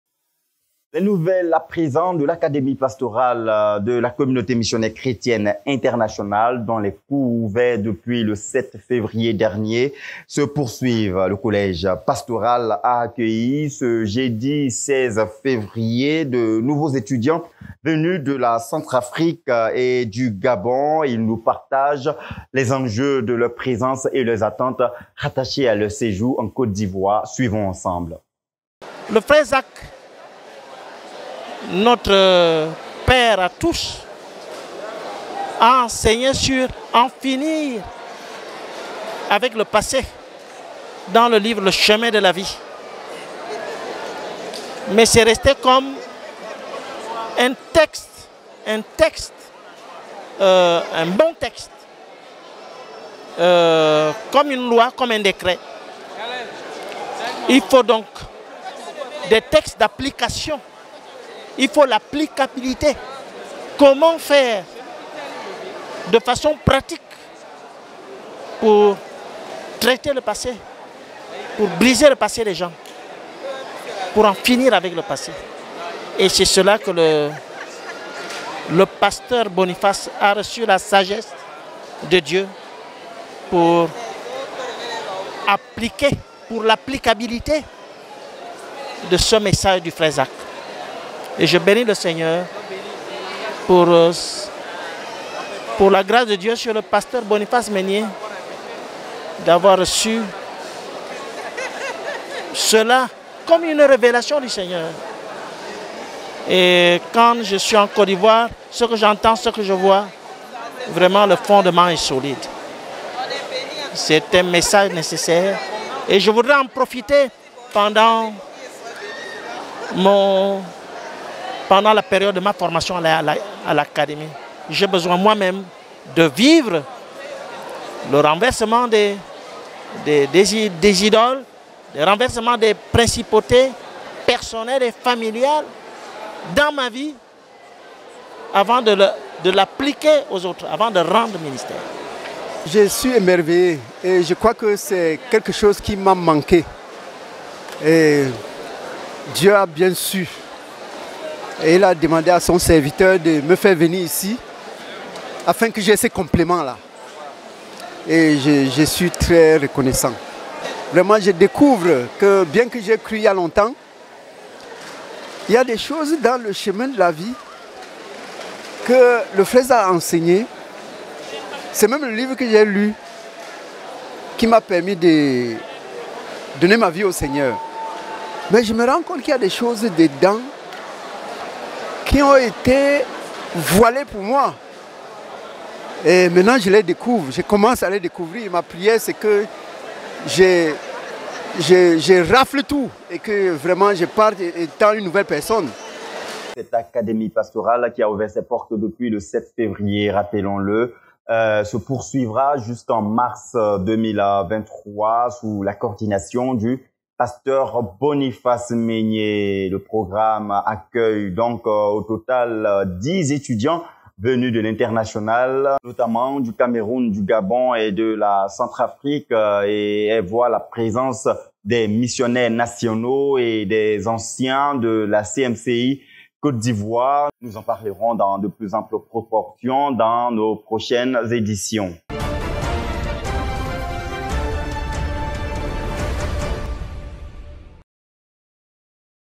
RTVC INFO - LES NOUVELLES DE L'ACADEMIE PASTORALE DE LA CMCI
Ouverte depuis les 07 Février Dernier l'académie Pastorale se poursuit tous les Jours jusqu'en Fin Mars 2023. Elle accueille en plus des anciens et des missionnaires de la CMCI Côte d'Ivoire, 10 Pasteur venus du Cameroun, de la Centrafrique et du Gabon.